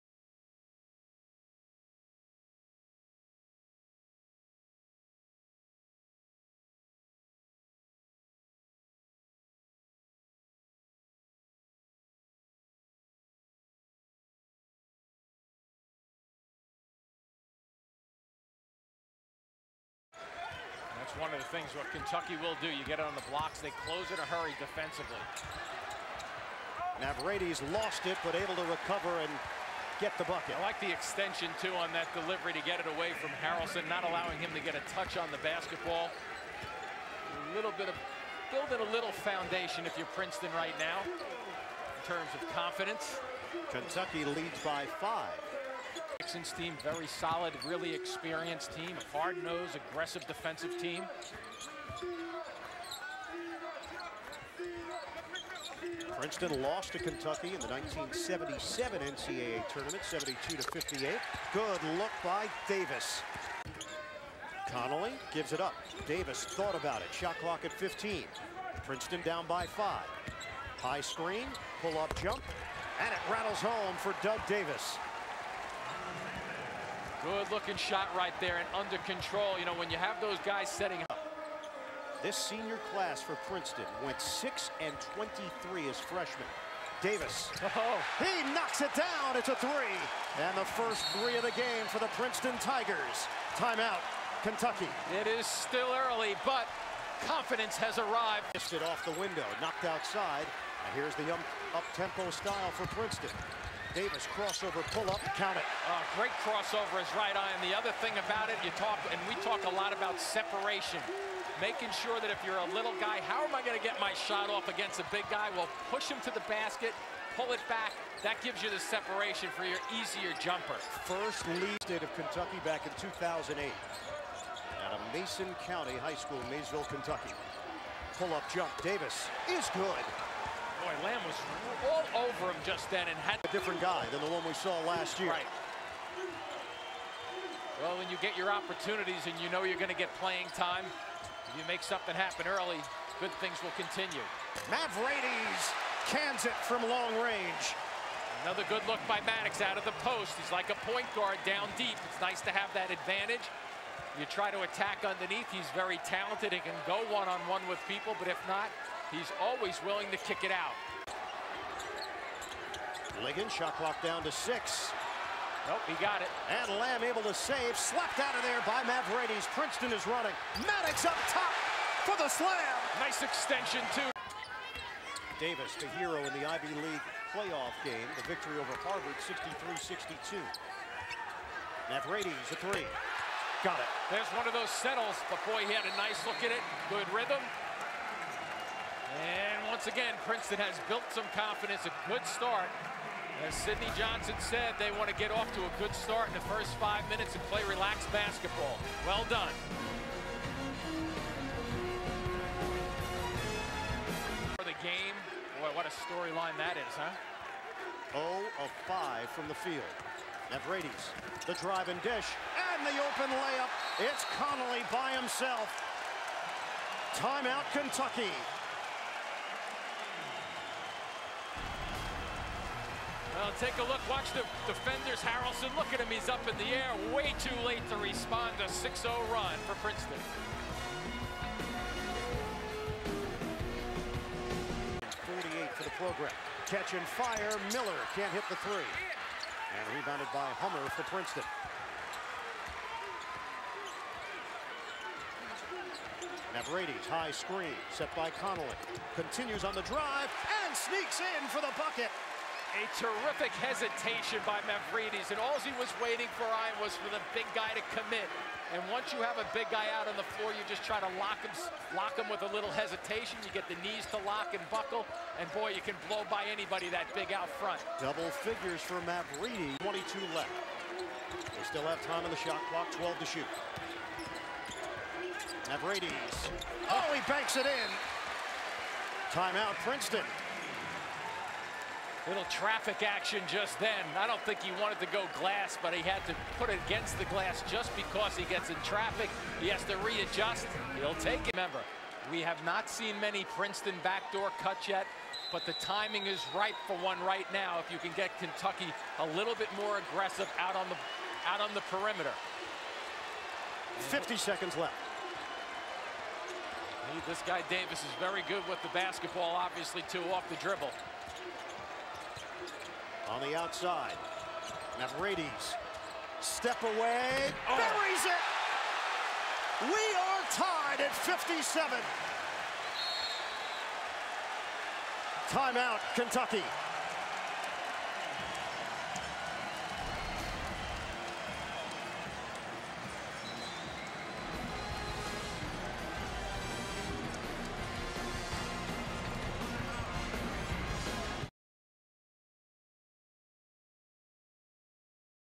2010-11 Doug Davis/Dan Mavraides Princeton vs. Kentucky
Highlights of Princeton backcourt Douglas Davis and Dan Mavraides in the 2011 NCAA Tournament game vs. Kentucky. Both players went over 1,000 points in their careers.

Video footage courtesy of CBS Sports.